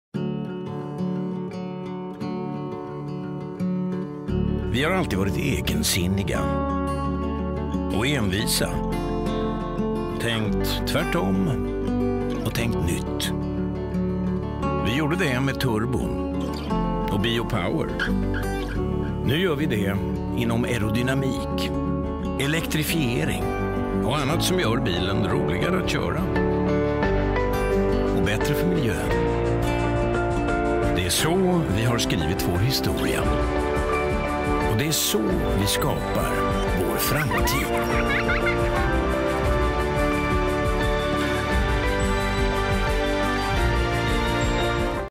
Saab TV reklam - Fortsättning följer
Senaste TV-reklamen från Saab. Filmens tema är "fortsättning följer" och illustrerar Saabs historia, den starka förankringen i Trollhättan och företagets framtidstro